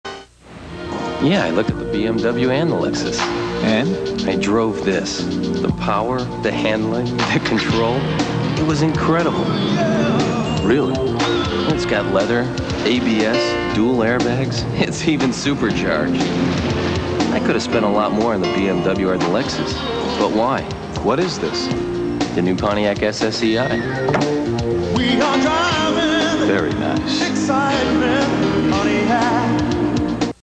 1993 Pontiac SSEi "I looked at the BMW and the Lexus" TV Commercial
The commercial aired in December 8th, 1992. It aired during the broadcast of the NBC News Magazine "Dateline NBC" on WKYC Channel 3 in Cleveland, Ohio.